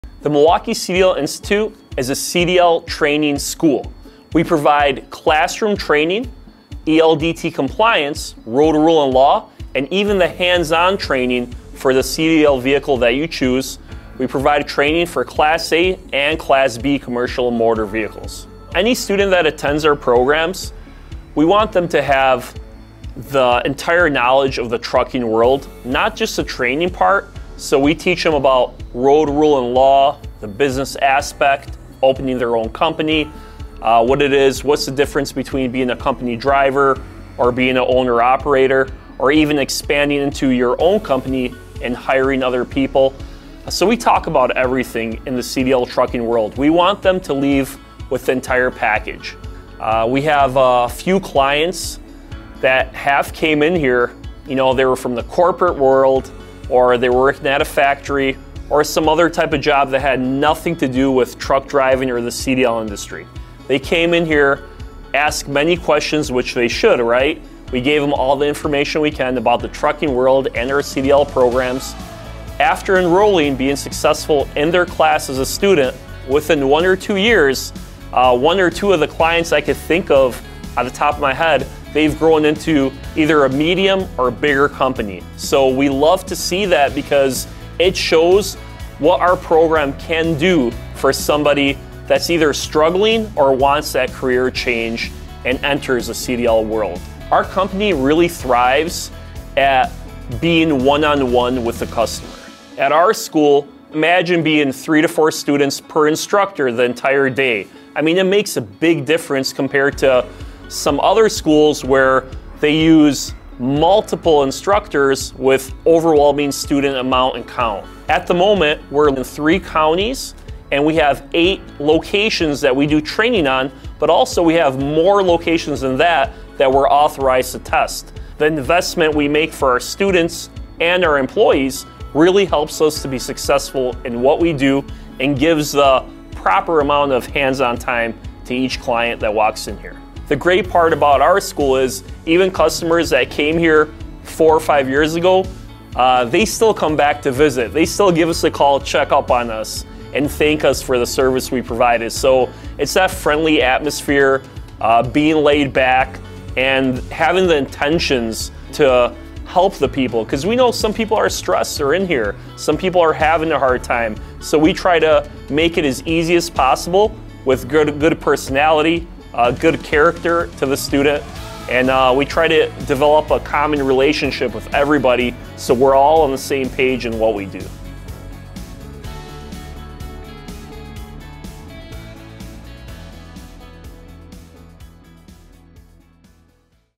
ABOUT US! CDL DRIVING SCHOOL CDL CLASS A CLASS B MILWAUKEE
THIS IS A LITTLE INTRO ABOUT US! WE TRAIN AND TEST FOR ALL 50 STATES!! JOIN OUR SCHOOL TODAY! MILWAUKEECDL.COM MYCDLBUDDY